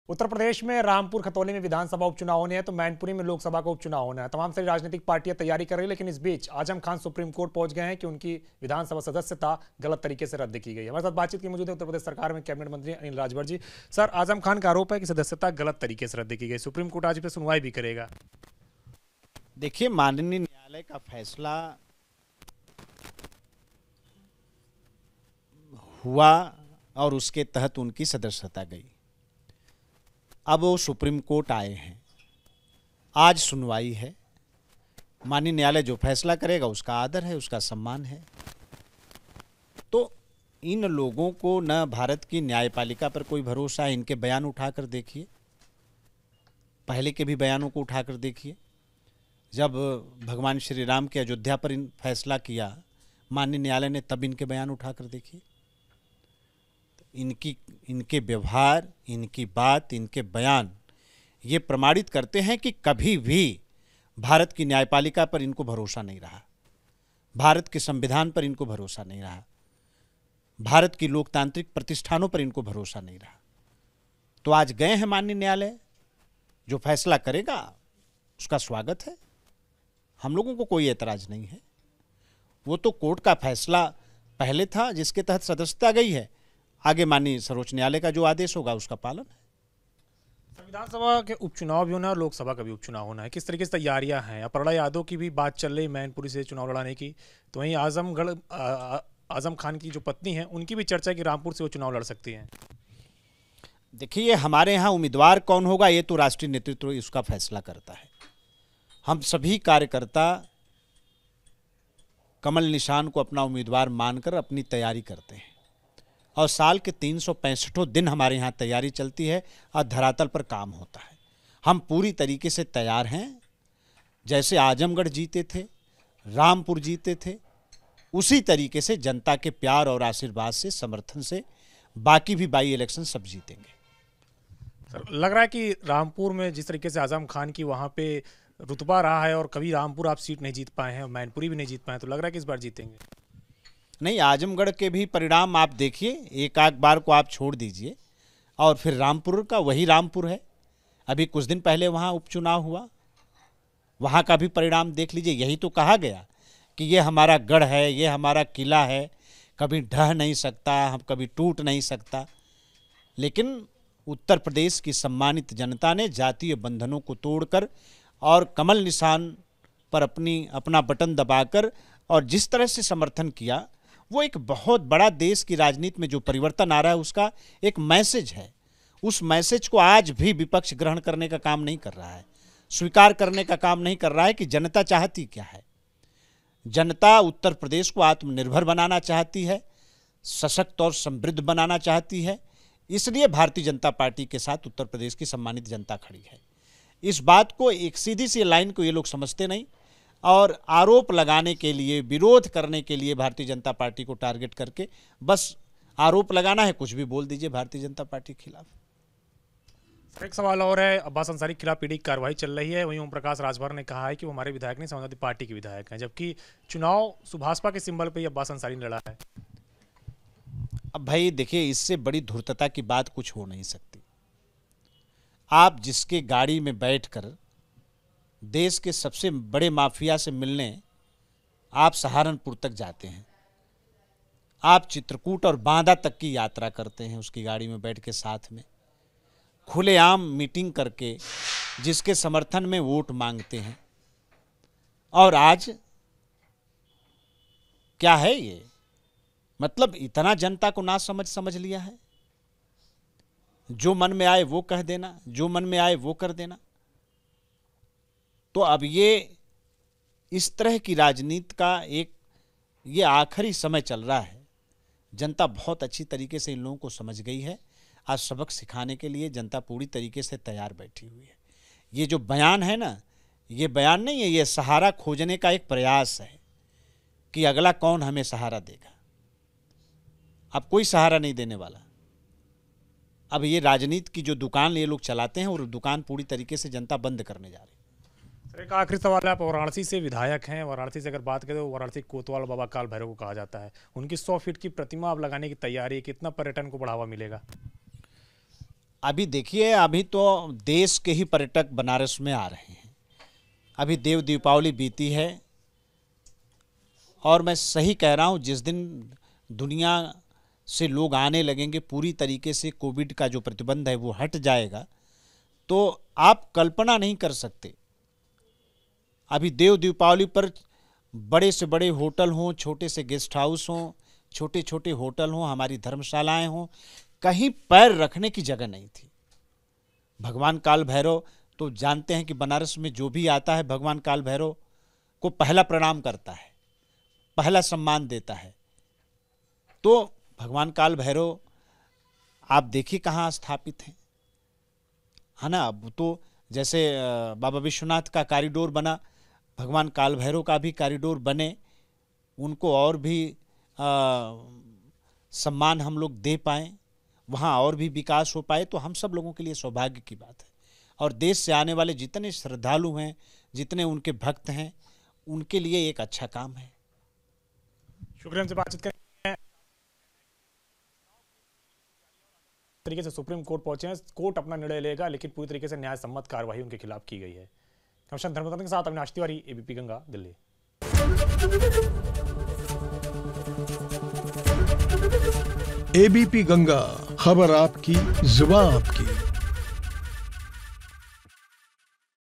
Rampur और Mainpuri भी जीतेगी BJP, पहले भी सभी के गढ़ों को ढहाया : ANIL RAJBHAR
यूपी सरकार के मंत्री अनिल राजभर का बयान- आजम खान के मामले में सुप्रीम कोर्ट की सुनवाई पर कहा उनका अधिकार है सुप्रीम कोर्ट जाना। लेकिन पहले भी कोर्ट के फैसले के आधार पर ही उनकी सदस्यता गई थी। जो भी फैसला आएगा वो स्वीकार होगा।
रामपुर और मैनपुरी भी जीतेंगे। हमारी पार्टी की पूरी तैयारी है। पहले भी सभी के गढ़ों को ढहाया है।